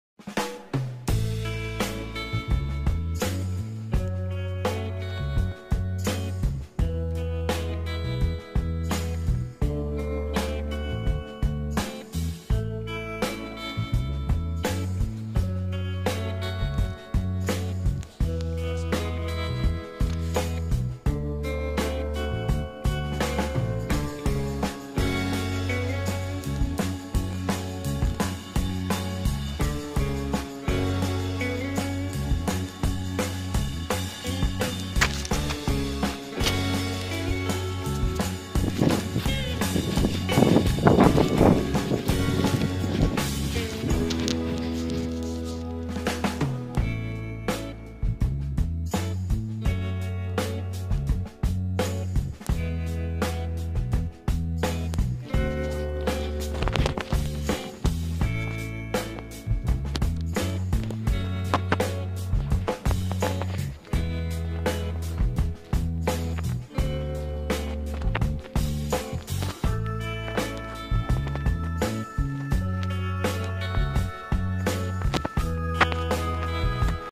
a much nicer view of rijeka,vitez and počulica
i hope you guys like the video it's about some villages and towns in b&h.